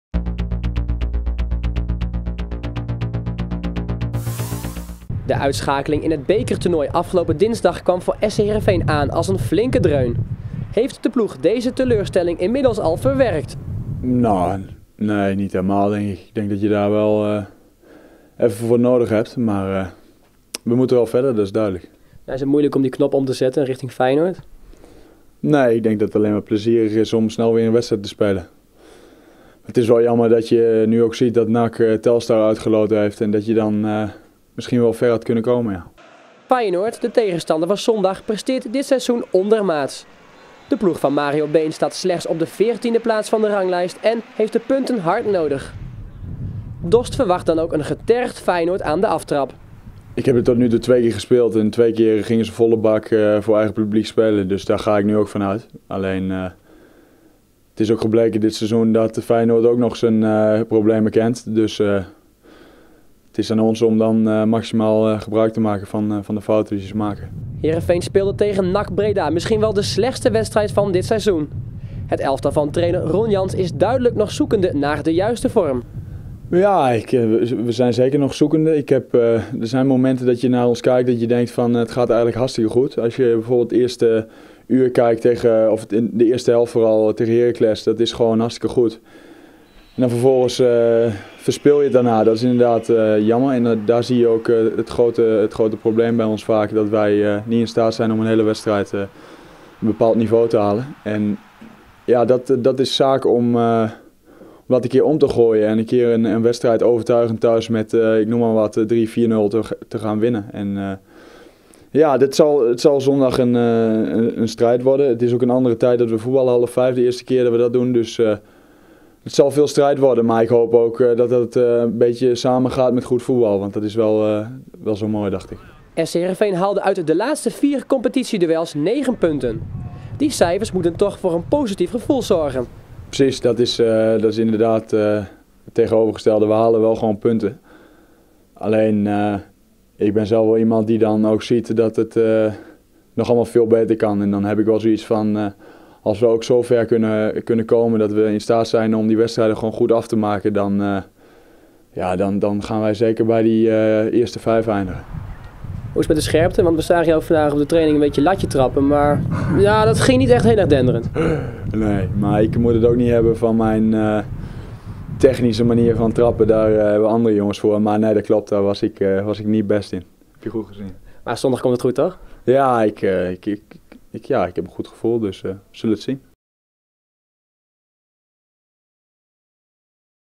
Voorbeschouwing Feyenoord - sc Heerenveen (seizoen 2010/2011)
Zondagmiddag speelt sc Heerenveen in de Kuip tegen Feyenoord. Bas Dost blikt vooruit op dit duel.